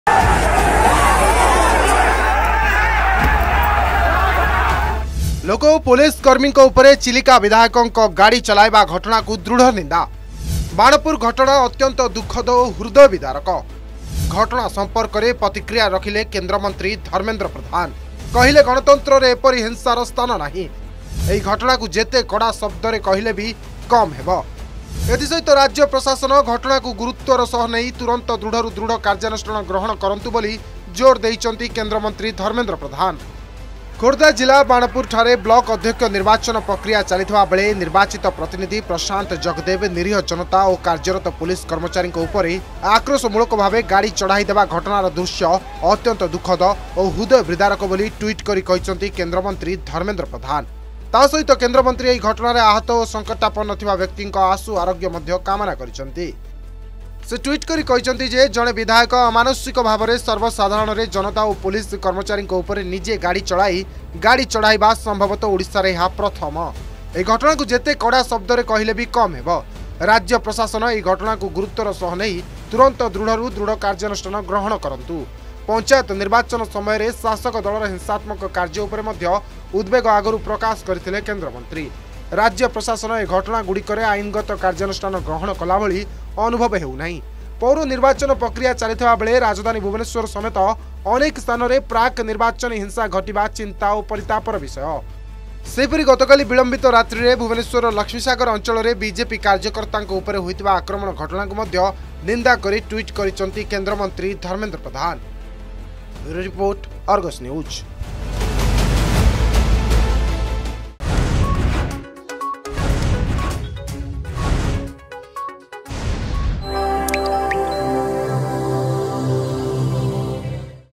Over 20 People Run Over by Car By Chilika MLA Prashant Jagdev, Dharmendra Pradhan Reacts Strongly
The Banpur incident is very tragic and heartbreaking. Union Minister Dharmendra Pradhan has reacted to the incident.